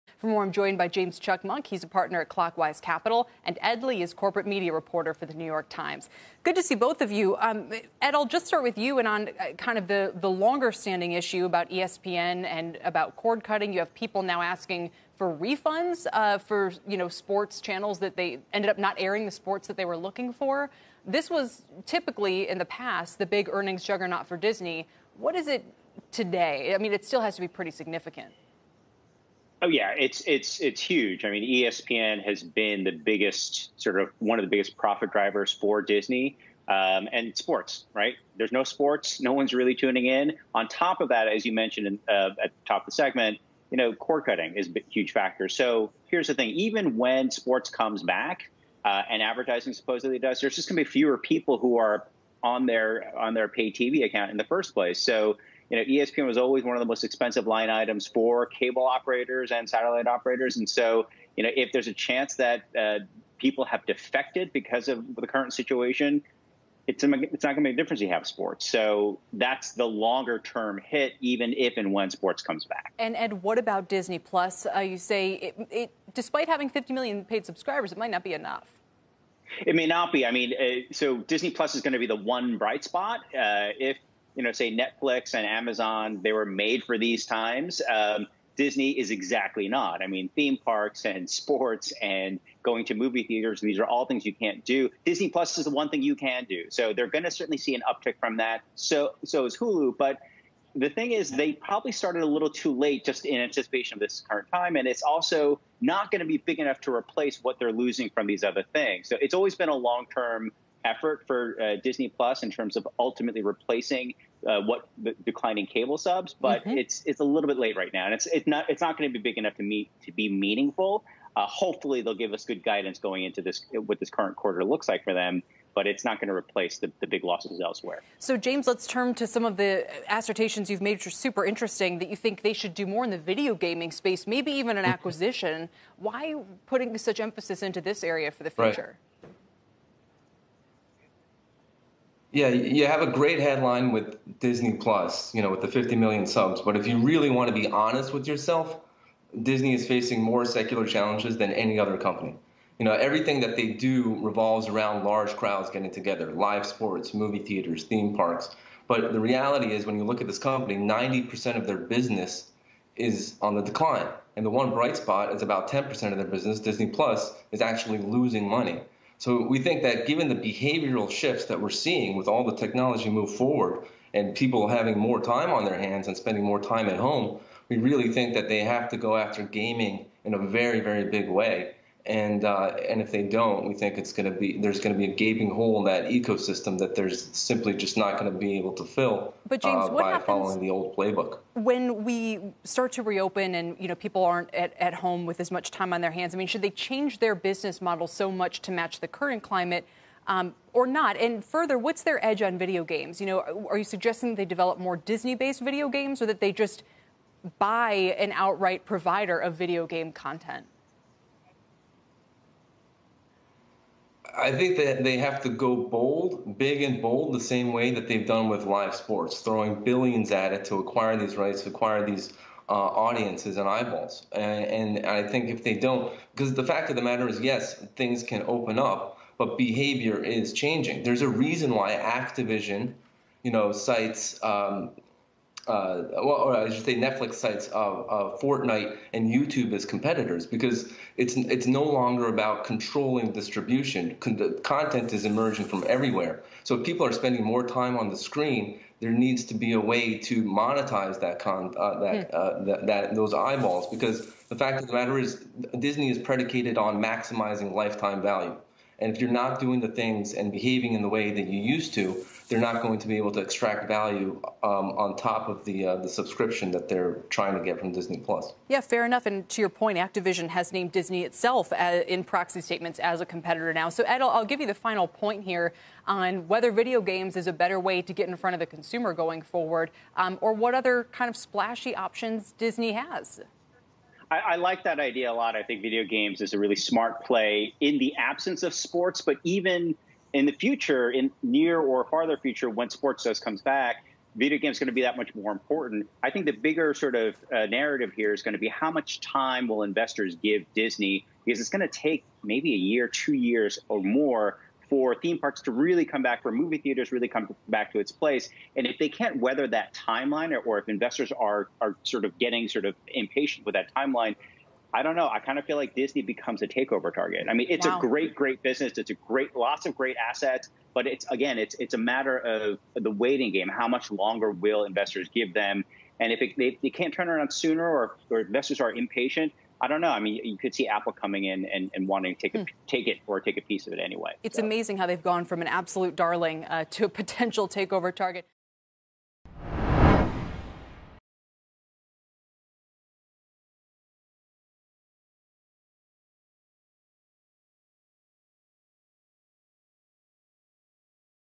Why this analyst says Disney likely needs major video gaming acquisition
CNBC's Kelly Evans discusses what investors should look for in Disney's earnings with Ed Lee of The New York Times and James Cakmak of Clockwise Capital.

CNBC TV